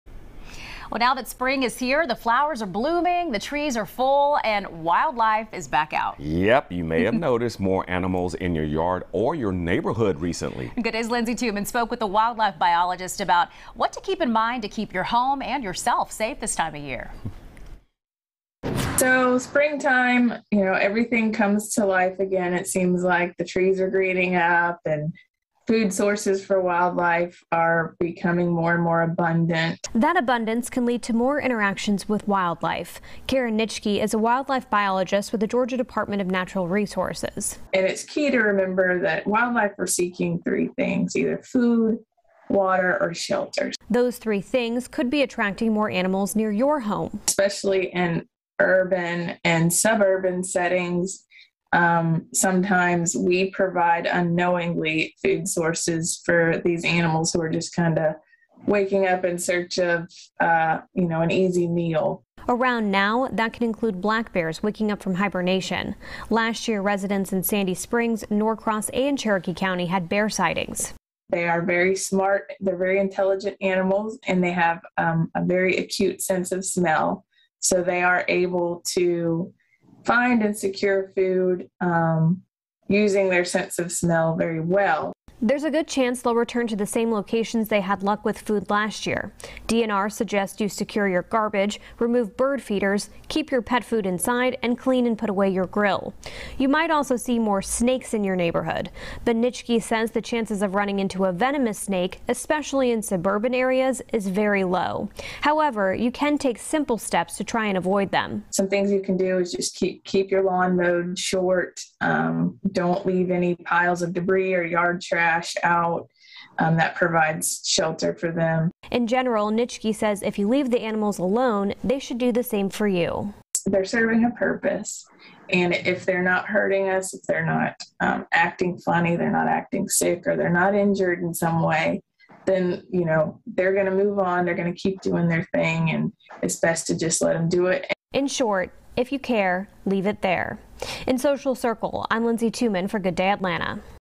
Dos and Don'ts for being around wildlife in the springtime
We talk to a wildlife biologist about things to keep in mind to keep yourself and your home safe this time of year.